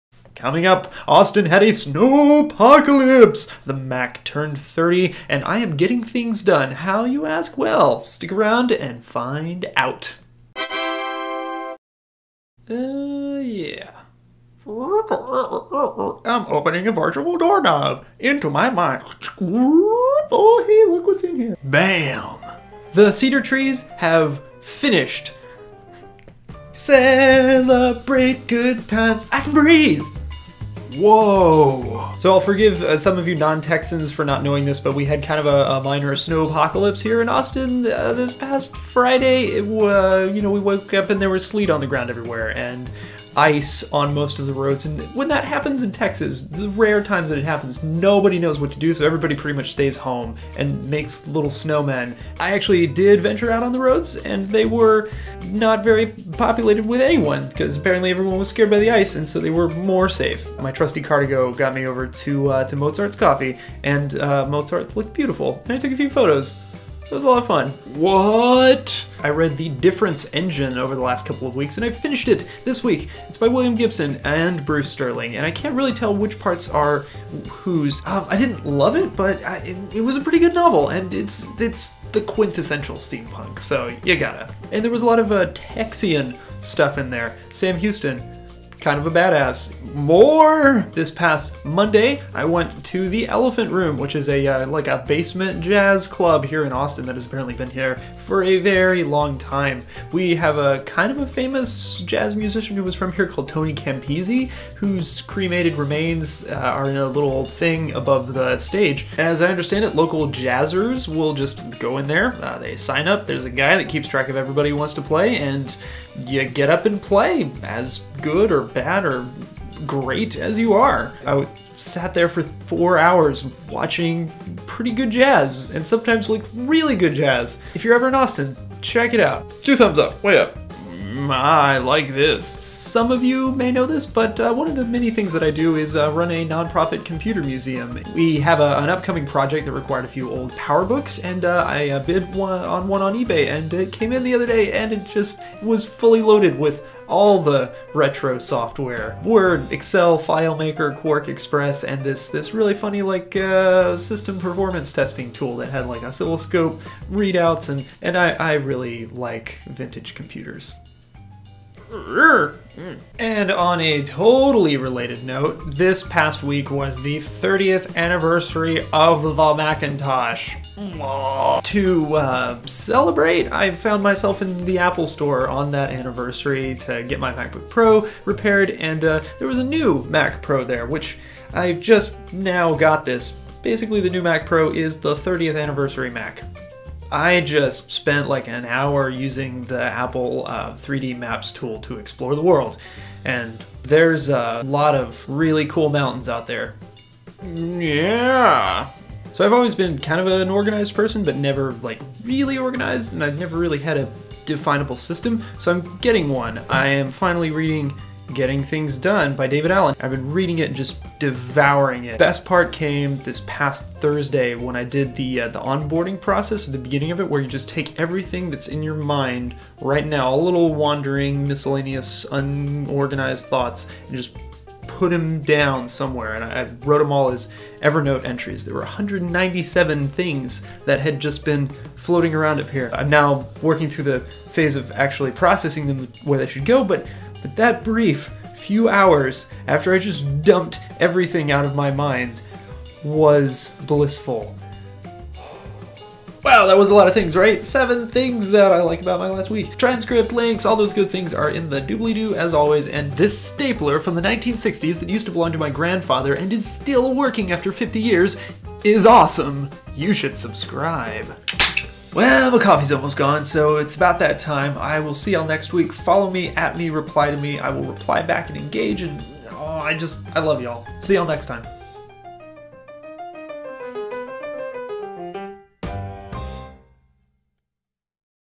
Week 4 - Austin Snowpocalypse! ...and More
Getting Things Done despite the Austin Snowpocalypse, thoughts on the Difference Engine, and more! It's been a busy week in my life.

## Show Notes

### 1. Cedar: DONE

(No links. It's just wonderful!)

### 2. Austin Snowpocalypse

### 3. The Difference Engine

The Difference Engine (Amazon)

### 4. The Elephant Room

### 5. PowerBook 170

### 6. 30th Anniversary of the Mac

### 7. Getting Things Done